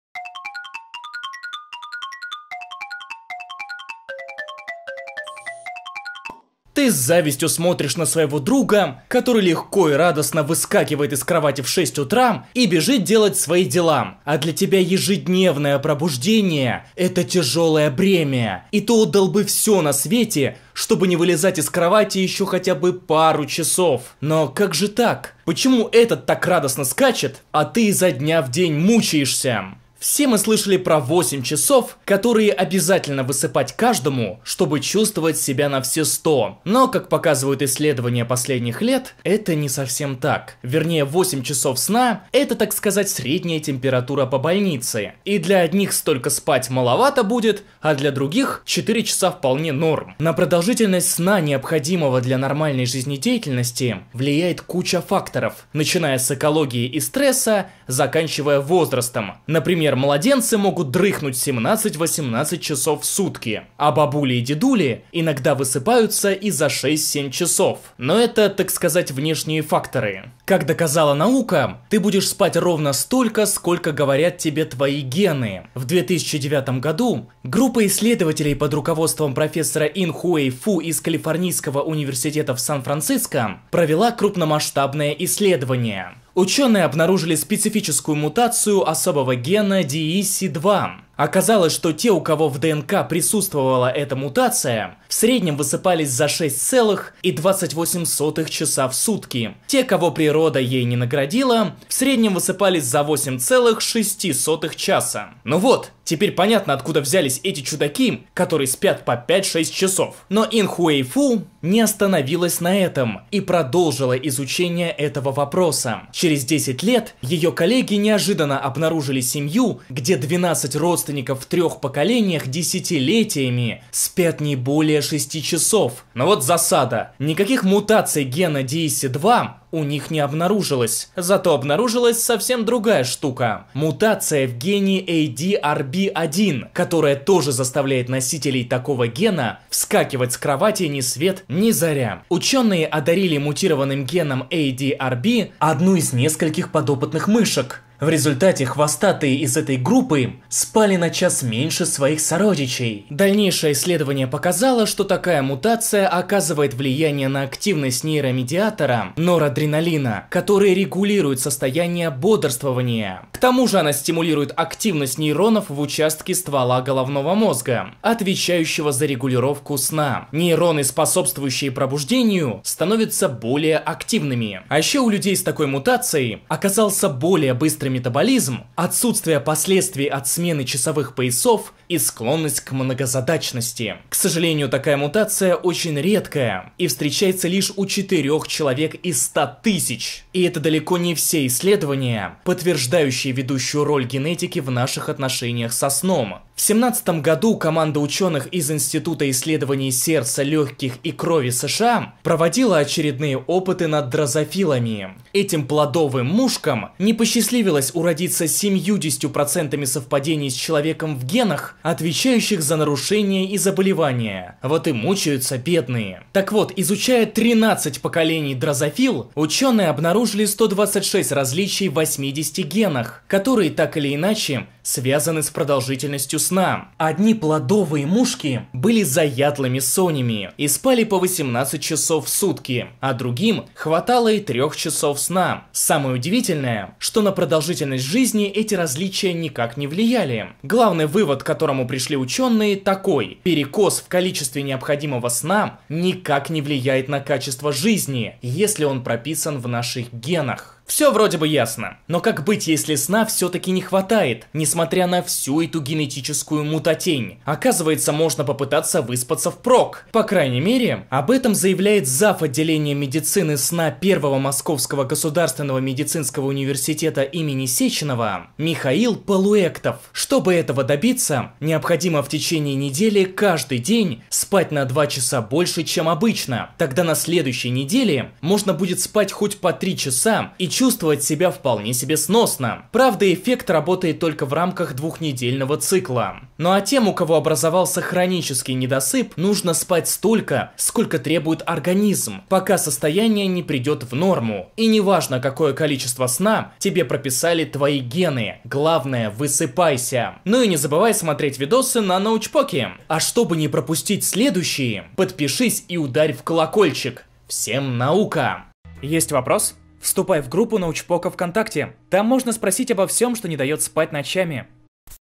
Как ВЫСПАТЬСЯ на неделю ВПЕРЁД? — Научпок
Очень многих (если не всех) мучает вопрос - как спать по 3 часа и быть бодрым? Ученые выяснили, почему люди не высыпаются... а если быть точным, почему кто-то высыпается, а кто-то хочет проспать всю жизнь. В этом выпуске продолжаем тему сна, как вы и просили :)

Автор сценария Наталья Швец

Смотри Научпок на Карамбе!

Вступай в нашу группу ВК!